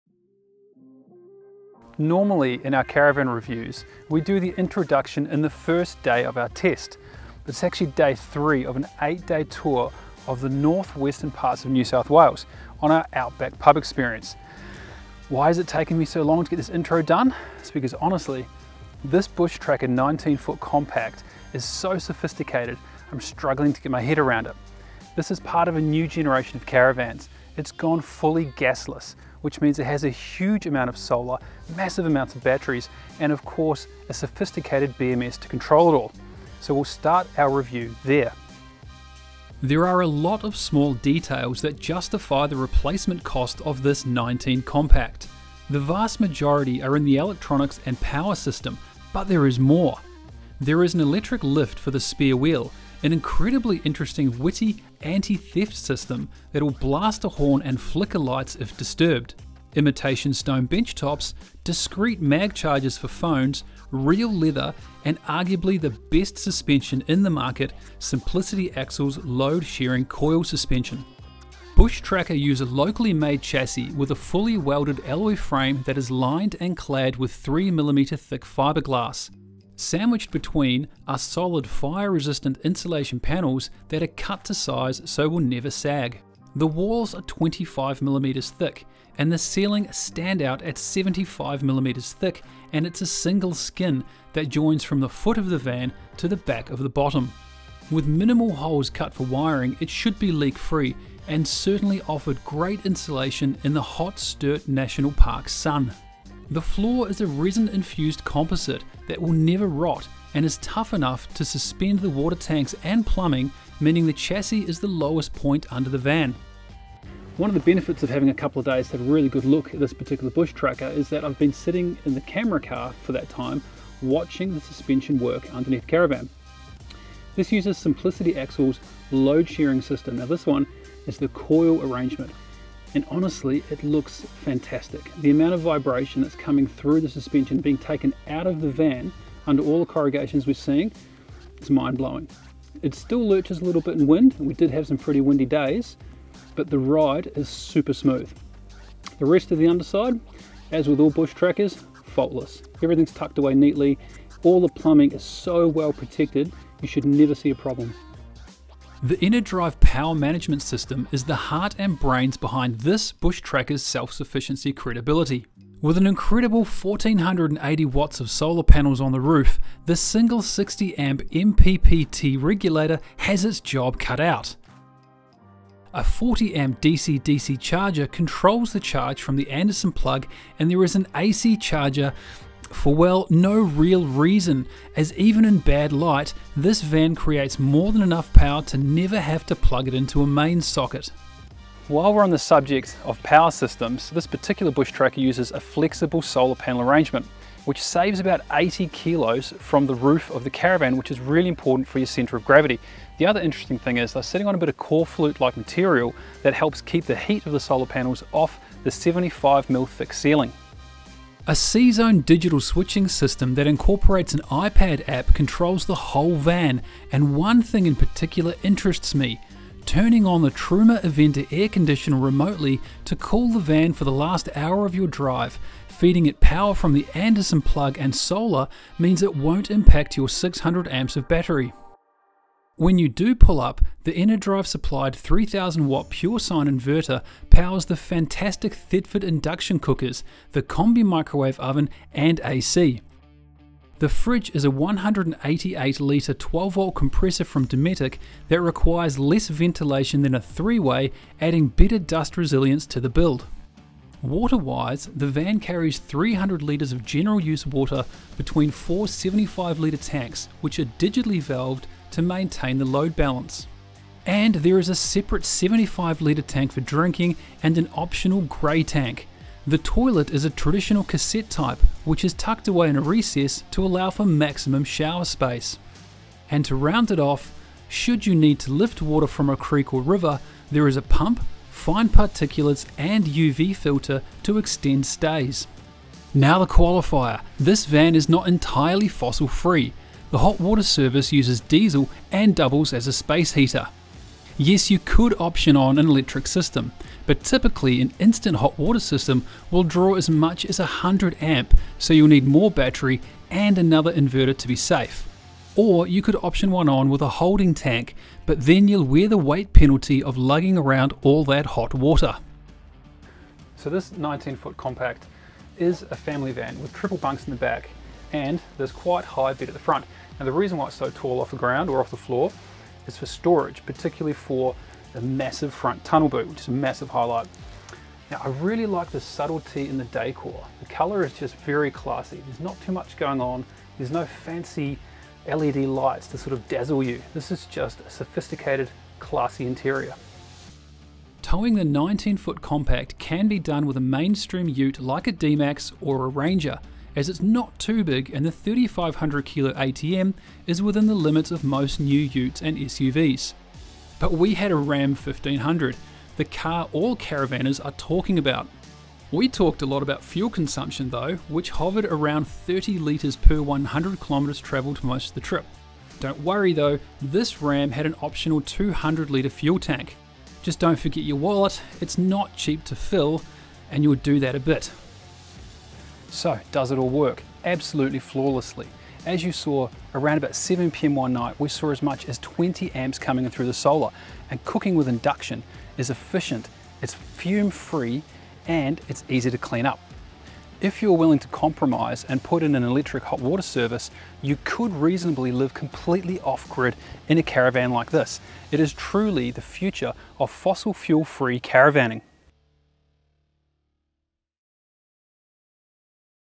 Bushtracker 19 foot family compact Caravan World review
Here is a video review from Tim at Caravan World on the latest 19 foot compact family van we have built for display (set in beautiful outback NSW)...

A few key features: Truly generator free self sufficiency with 1.5KW solar and 600Amps of lithium, gas free internal & external cooking, Simplicity Axles load sharing coil, 2 x electric awnings, smart controls including automatic tank control through a touch screen and heaps more...